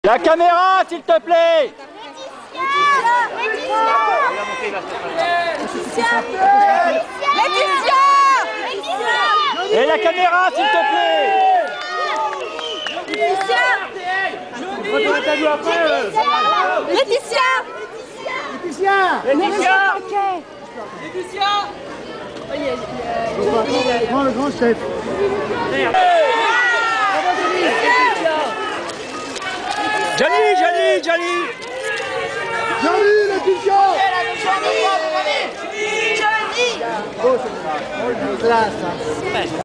Johnny Hallyday et Laetitia - Mariage de Mimie Mathy 27.08.2005
Johnny Hallyday et Laetitia au Mariage de Mimie Mathy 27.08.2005
Mairie de Neuilly (johnny hallyday est témoin de Mariage de Mimie Mathy)